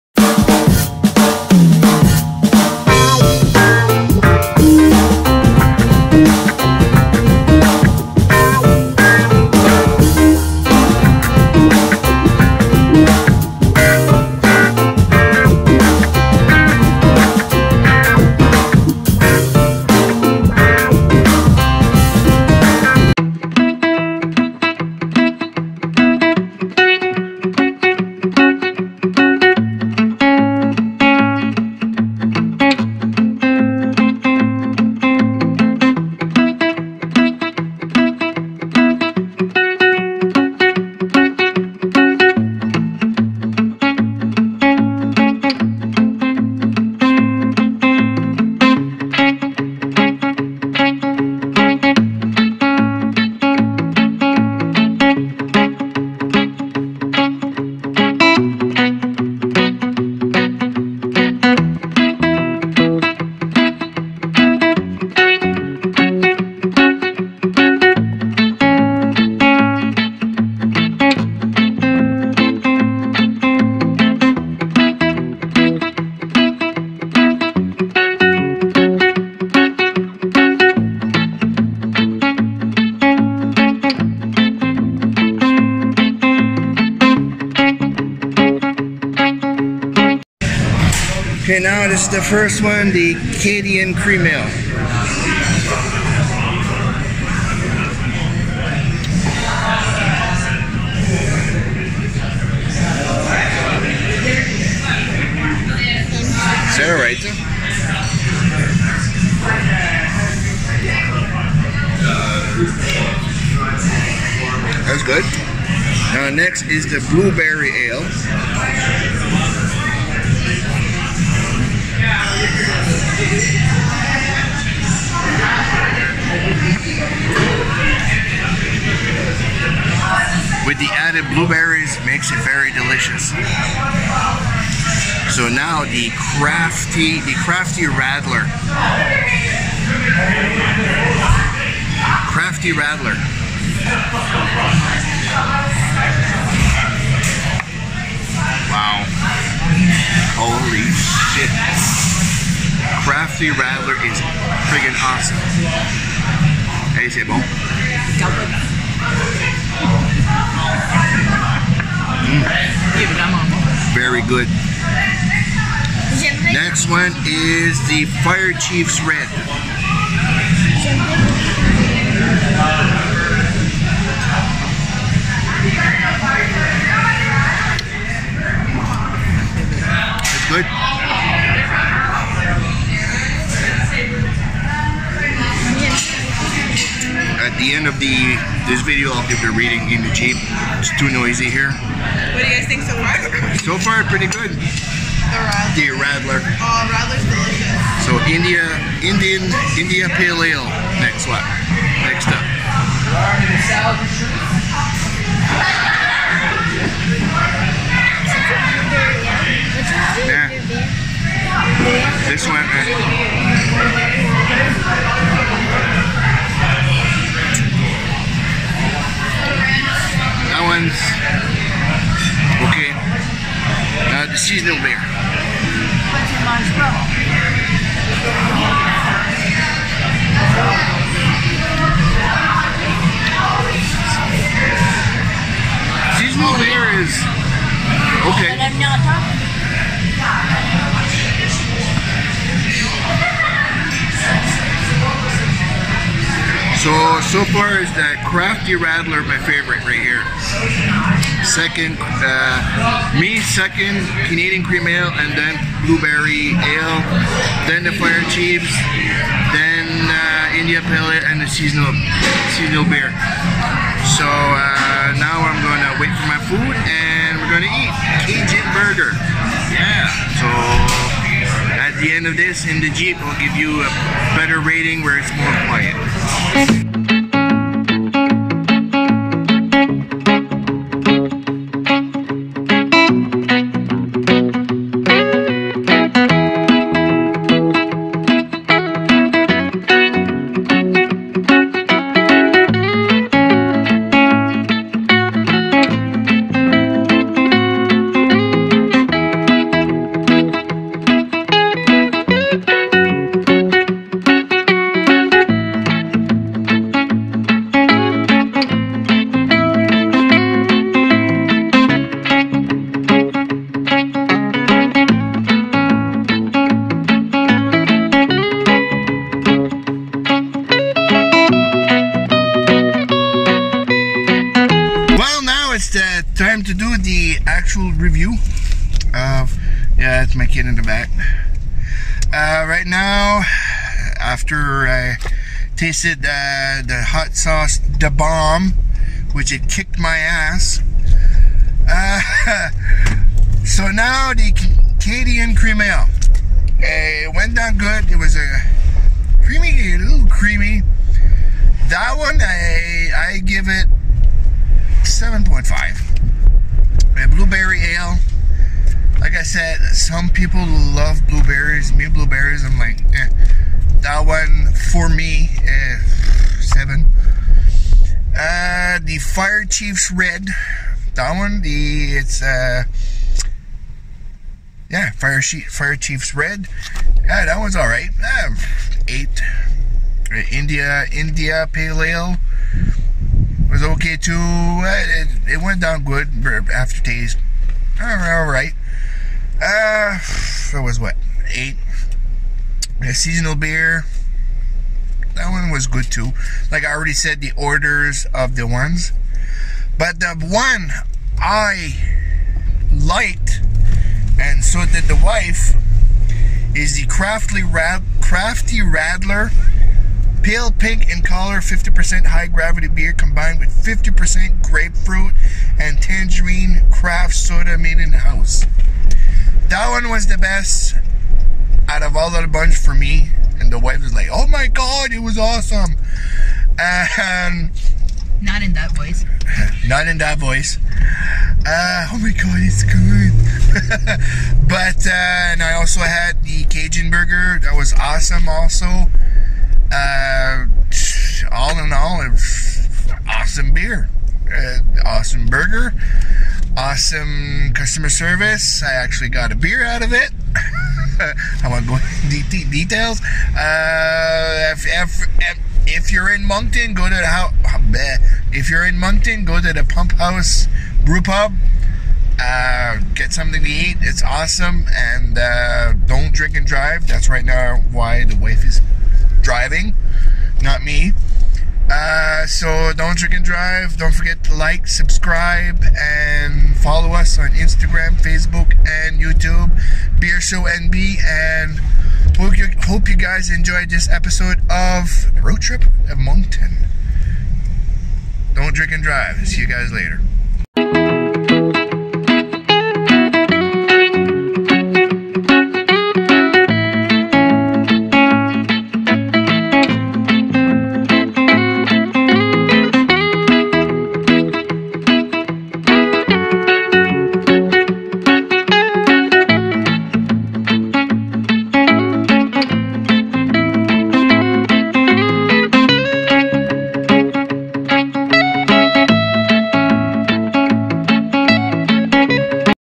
BEER SHOW NB ROADTRIP PUMPHOUSE BREW PUB
Today i visit pump house bree pub in moncton nb awsome food and awsome beer.